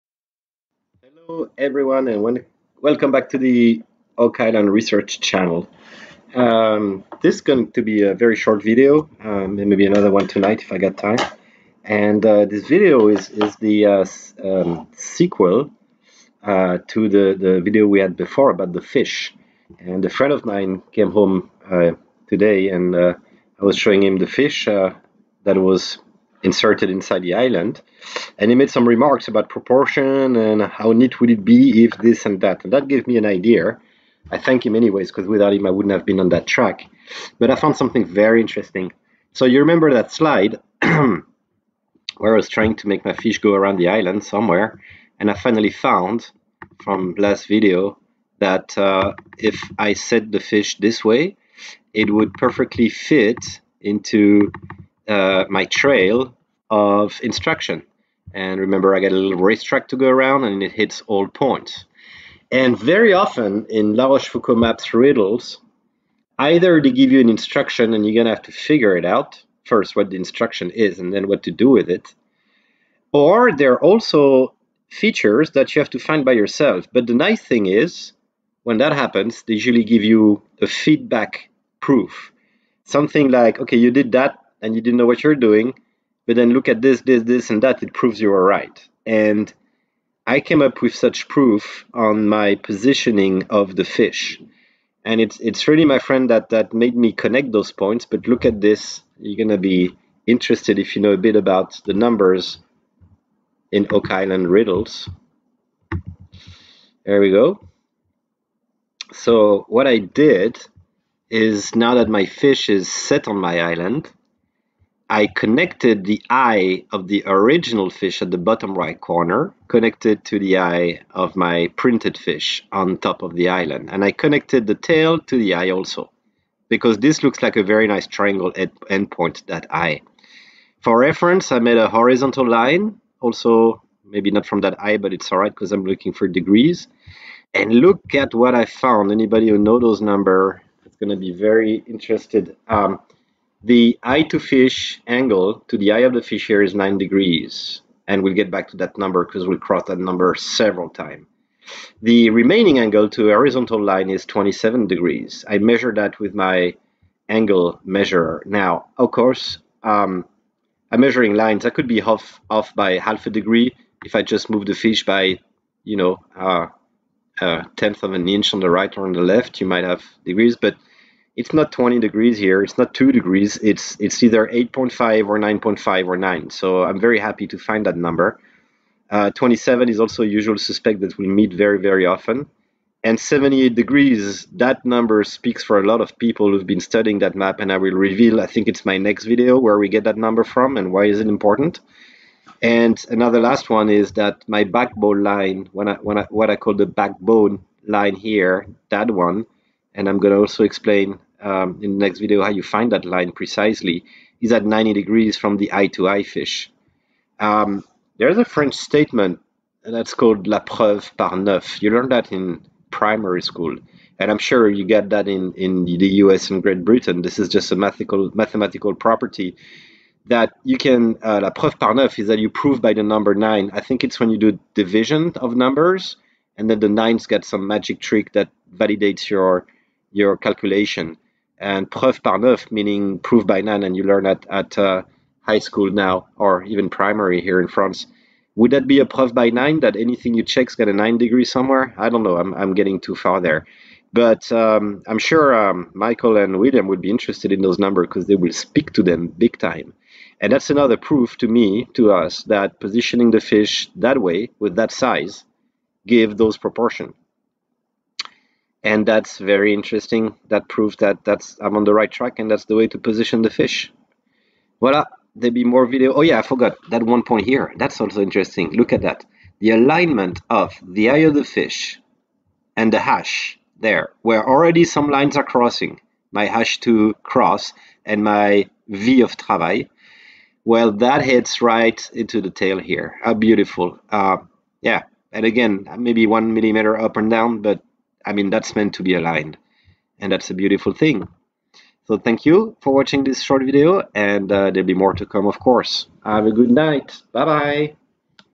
5 - OAK ISLAND - THE SEA BASS HAS AN ANGLE !
LA ROCHEFOUCAULD MAP
We are going to find confirmation that we are on the right track with out fish on the island.